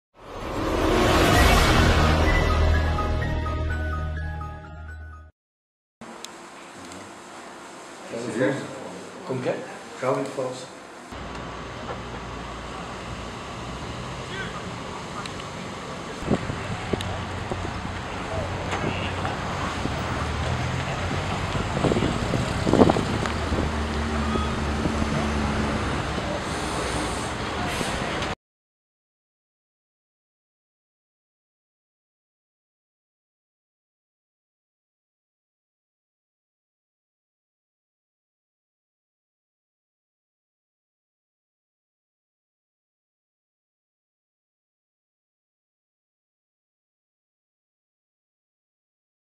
Homem é preso com carro roubado
Homem é preso em Araucária com carro roubado. A prisão ocorreu em uma ação conjunta entre serviço reservado da PM e GM, que após uma perseguição na rua Marcelino Jachinski o interceptou, o mesmo acabou colidindo com outros veículos na via